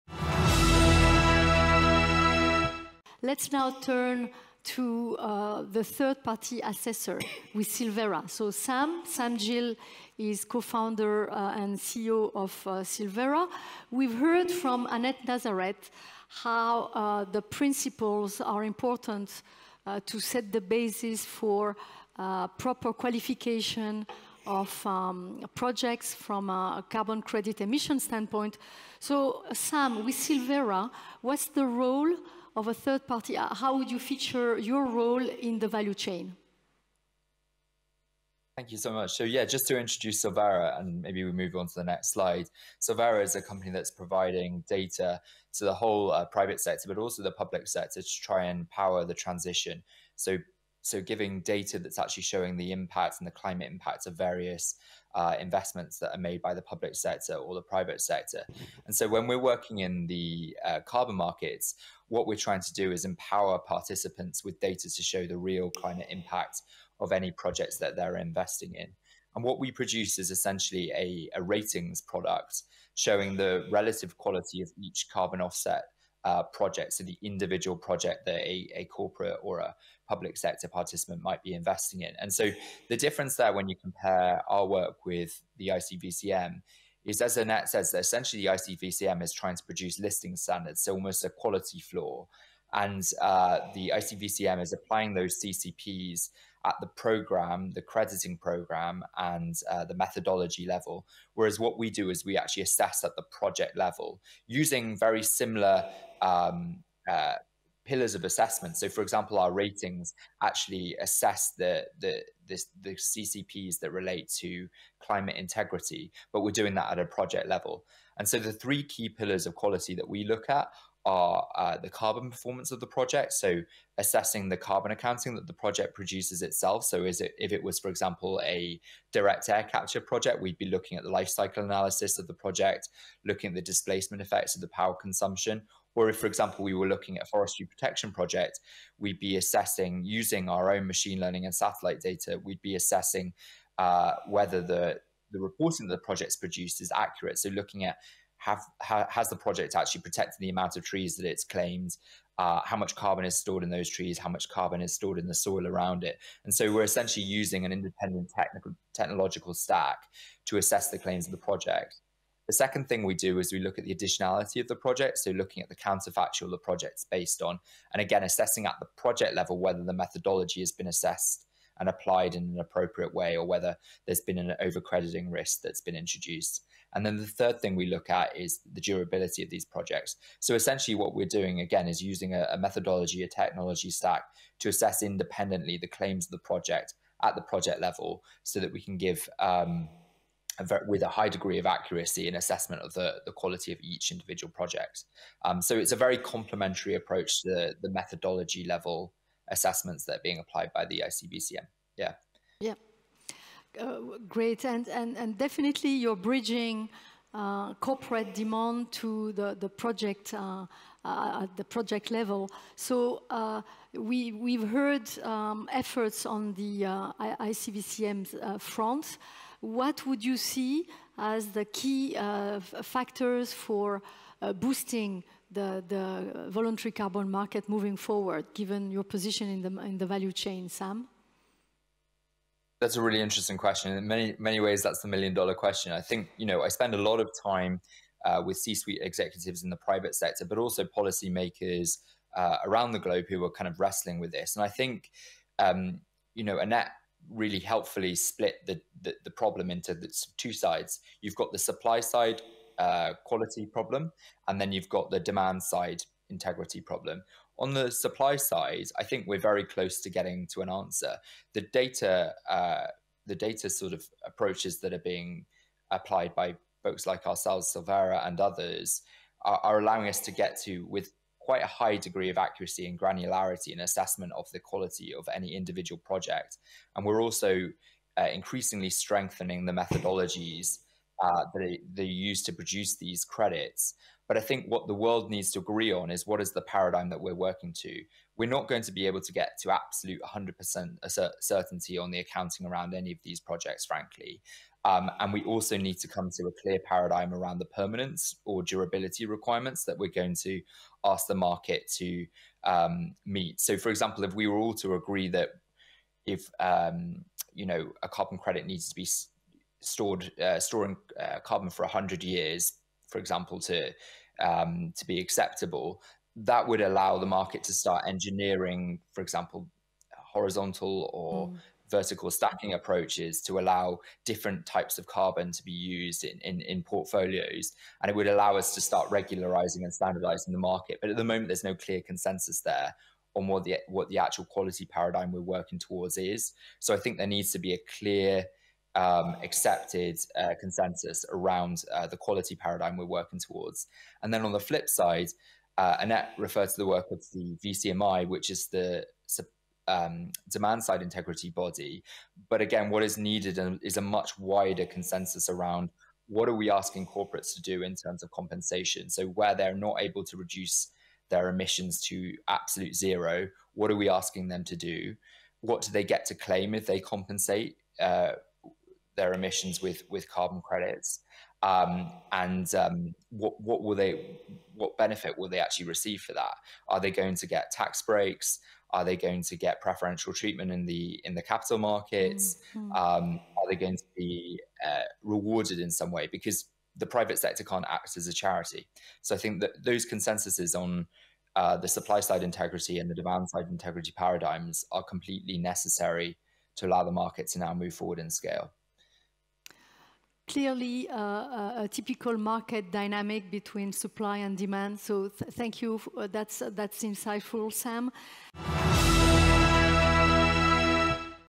WPC 2023 - Sam Gill - Session 12: How Can Carbon Credit Contribute to Net Zero?
Sam Gill is the Co-Founder and President of Sylvera